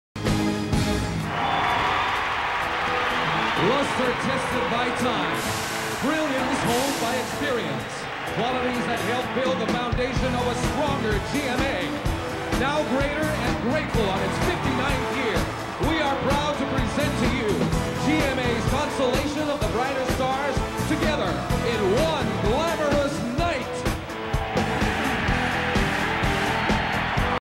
Vanishing Curtain - Stage Riggers - GMA Anniversary.wmv
GMA 59th anniversary celebrated at Araneta Colesium. Trusses, Rigging and special reveal by Stage Riggers Inc.